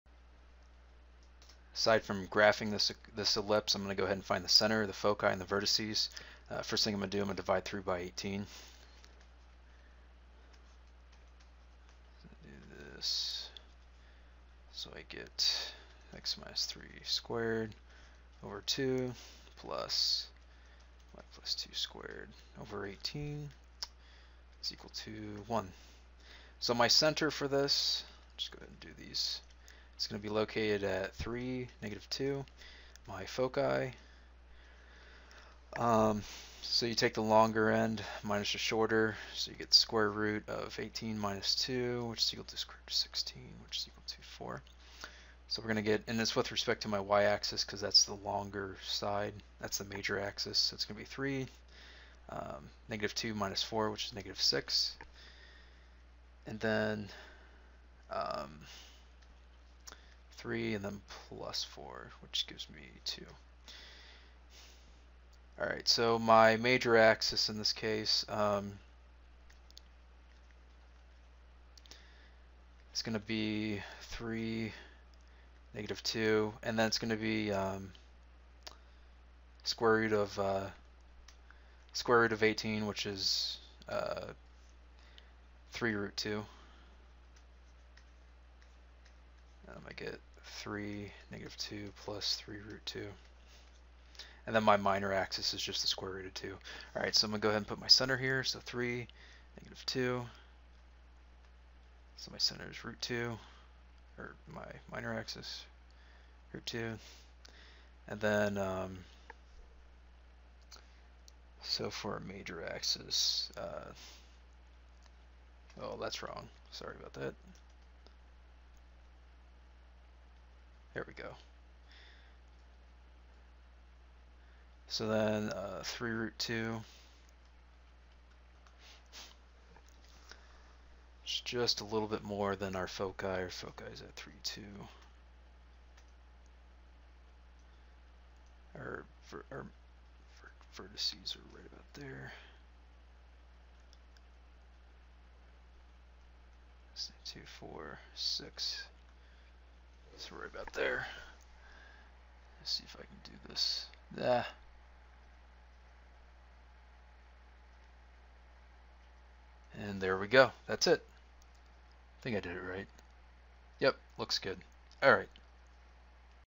9(x-3)^2 + 9y+2)^2 = 18 For the ellipse find the center, foci, and vertices, graph the equation.
Find the properties of the ellipse and then graph it.